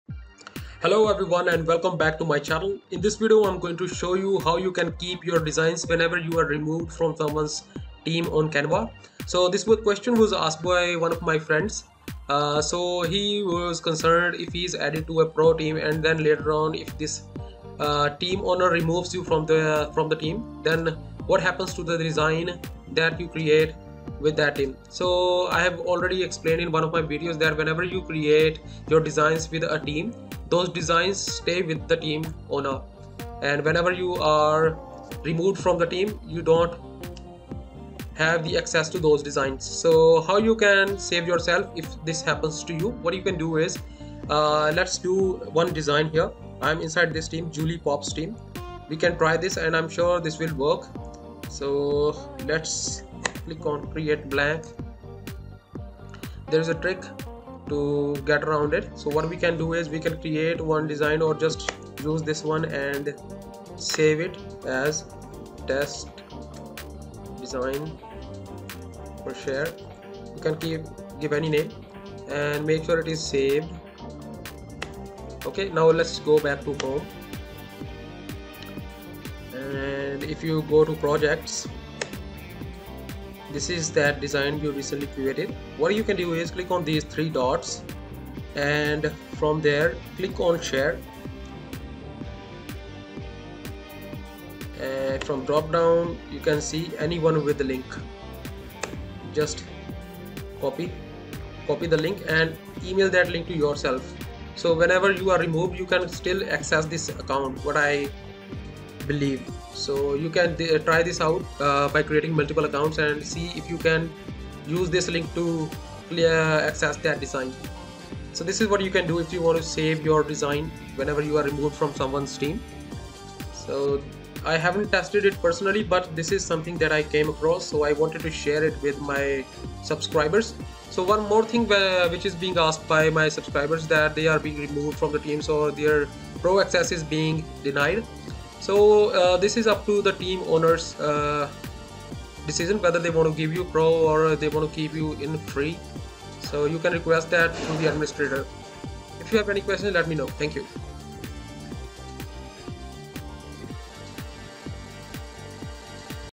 Sharing Canva Pro Team Designs With Anyone - Keep Your Designs Even When You Are Removed!
Try using this trick to keep your Canva Pro Team design even if you get removed from the pro team.